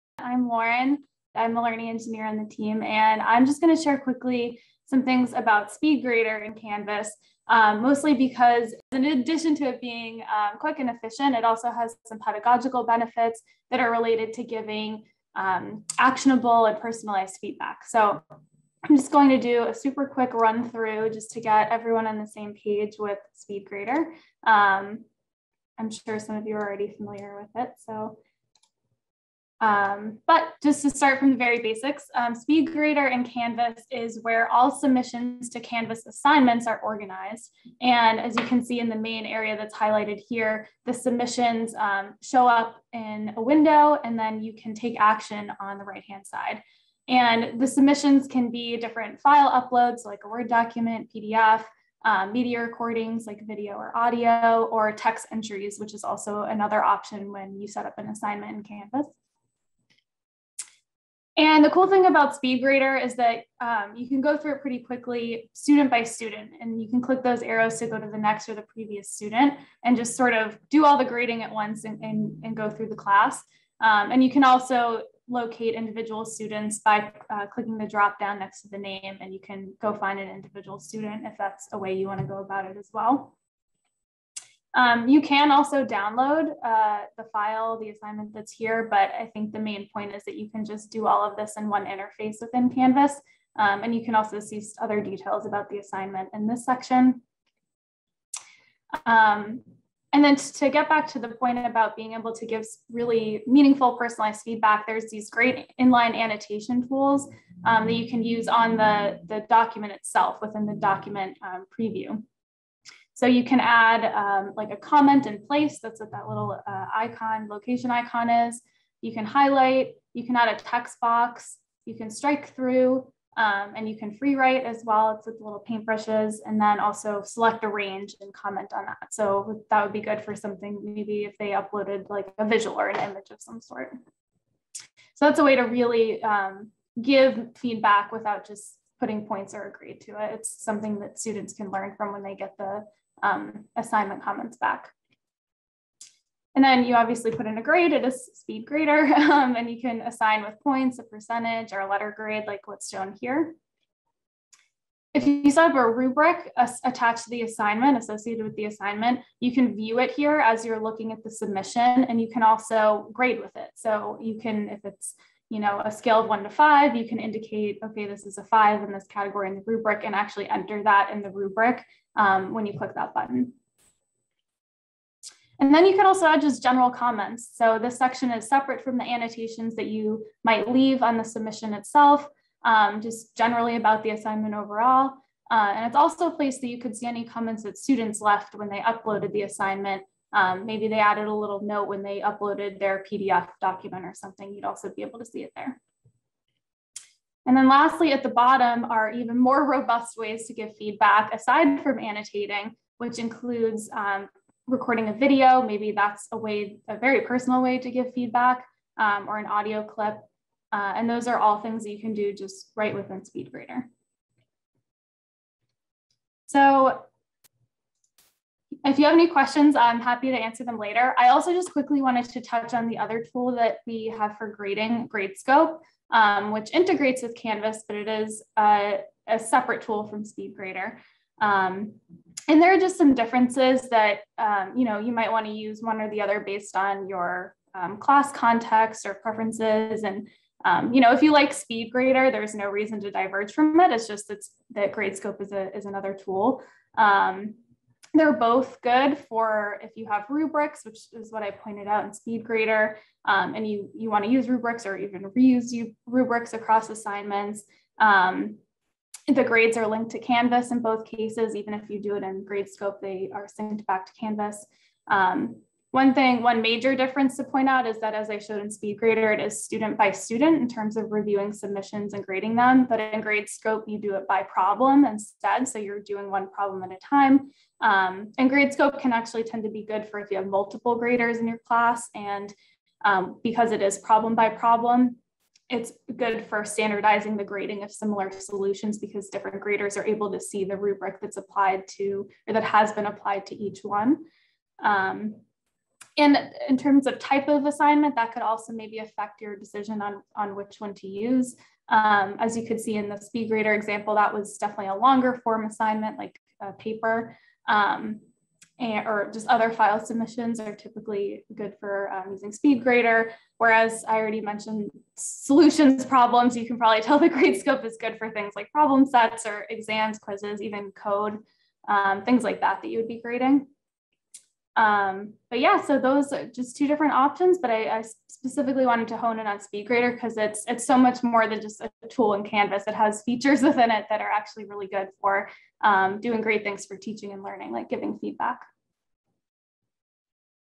Grading in Canvas: SpeedGrader
In this video, Learning Engineer Lauren Totino gives an introduction to the SpeedGrader tool in Canvas -- its functions and features. Totino also contrasts SpeedGrader with Gradescope, illustrating the relative strengths of the two.